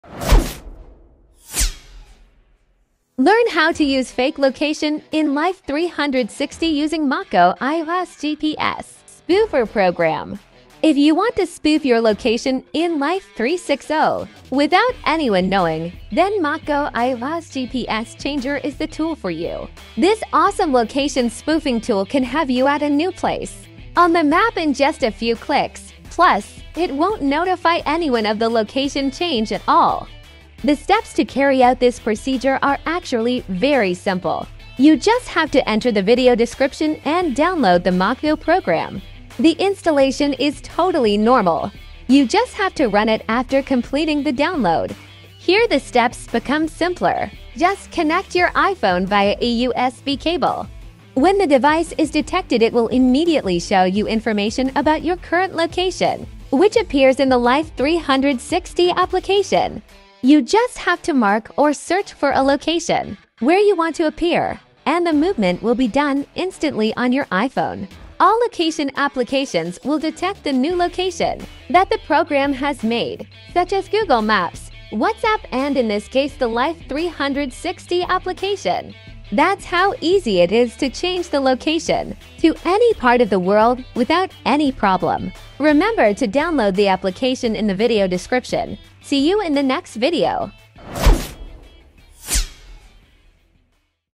Fake Location on Life360 on an iPhone, iOS 17 Full Compatible [2024]
Today, we're going to talk about a topic that has been generating a lot of buzz lately - faking your location on Life360 using Foneazy MockGo. You may be wondering why someone would want to fake their location on Life360. Well, there are a few reasons. Some people want to maintain their privacy and not share their real-time location with others. Others may want to avoid certain restrictions or simply play a harmless prank on their friends or family. Now, when it comes to faking your location on Life360, Foneazy MockGo is a popular choice. It's a user-friendly tool that allows you to spoof your GPS location without any hassle.